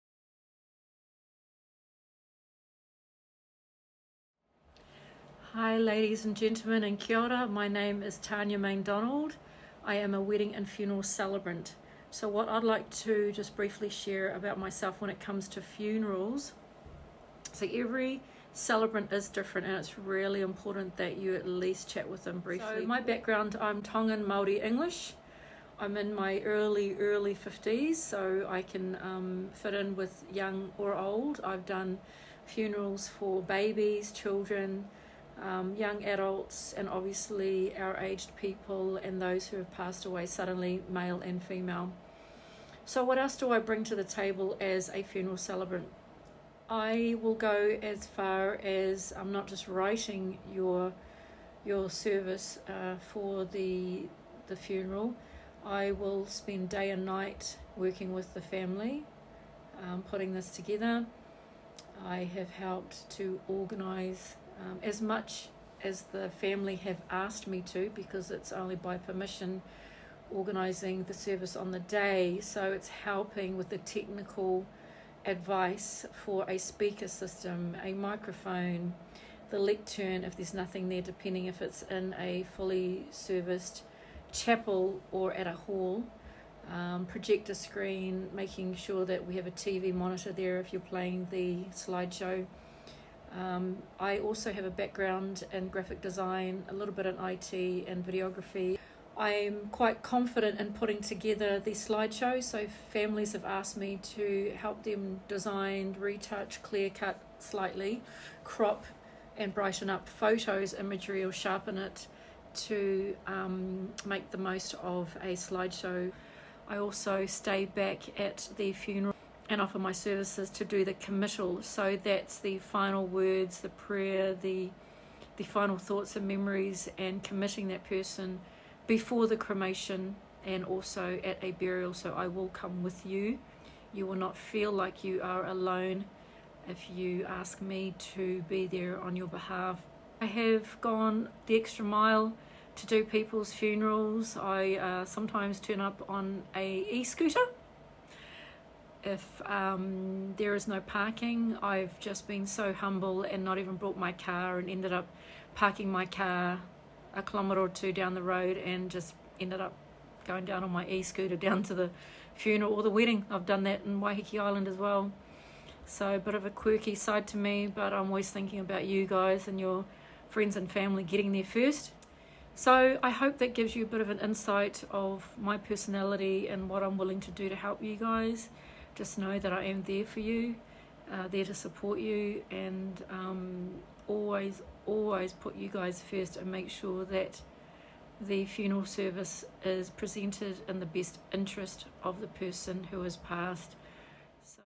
Tania Maindonald, What I offer as a Funeral Celebrant - YourNZCelebrant.com
Excited to share my first post and what came to mind is what I am passionate about the most which is officiating Funerals here in Auckland, New Zealand. I average 10 funerals a year even around the Covid pandemic challenge. I am based in West Auckland but am happy to travel to great Auckland areas if asked. I am Tongan-born, Maori and English and have been a Celebrant for over 5 years and hold some of the most Google Reviews as a Celebrant in New Zealand. I don't take that lightly, it comes from working hard and doing what the family have asked for but as you will see, I go that extra mile and hope my passion shows in what I do. If you see my tagline on my homepage of my it says "Professional is not a label you give yourself - it is a description you hope to earn from others."

Enjoy my informative video on what it takes to be a funeral celebrant and other package deals I offer for this type of service - enjoy.